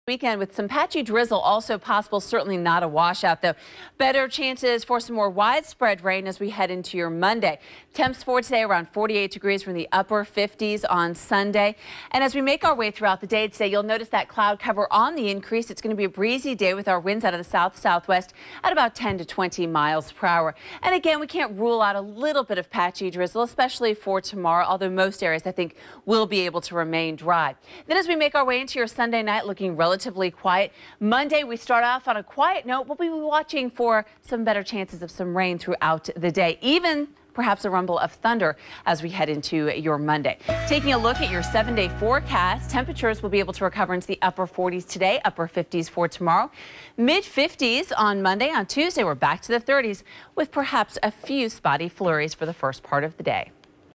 MIlder Weather This Weekend
WLKY meteorologist Susanne Horgan has a look at your forecast.